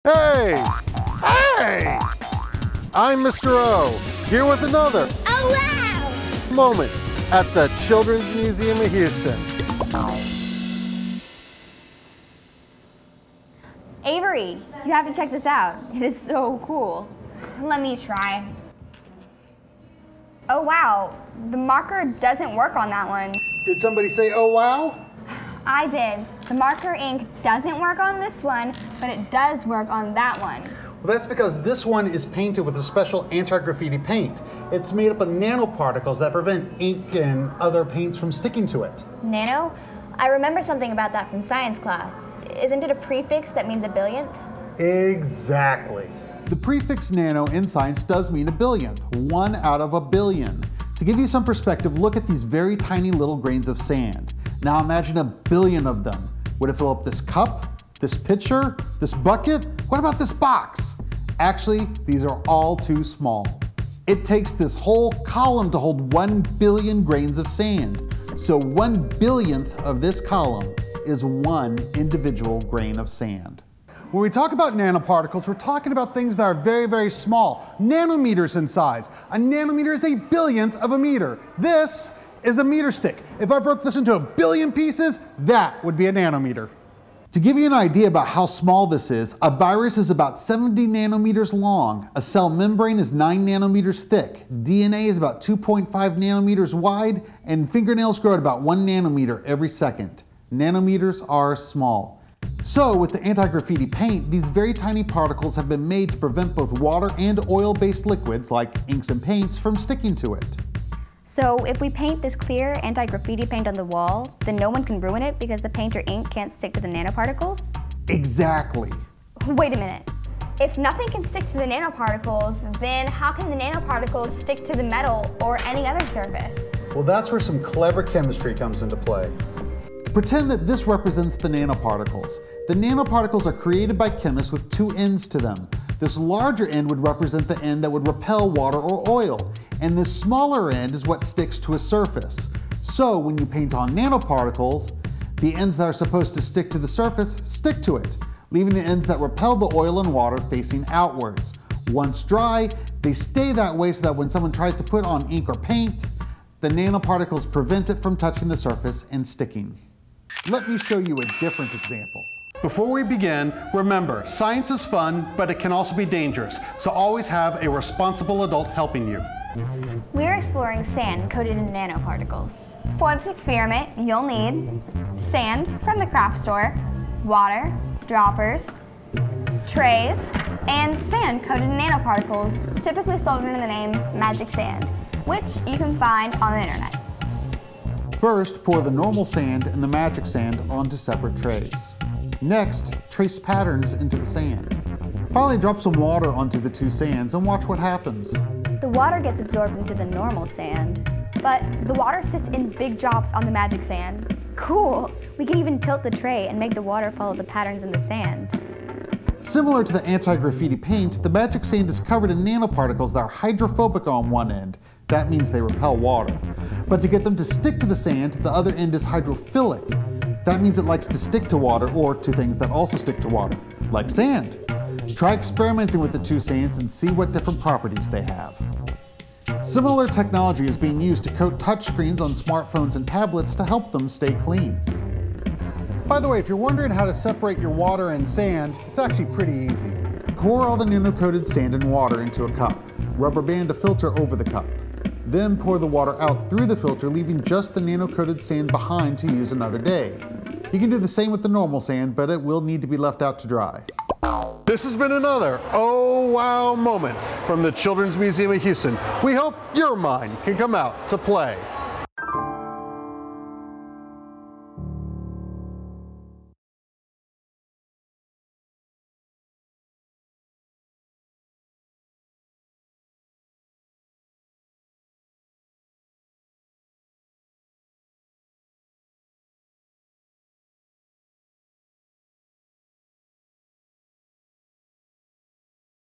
Nanocoatings
In this episode of O Wow Moments with Mr. O from the Children's Museum of Houston, we take a look at Nanocoatings!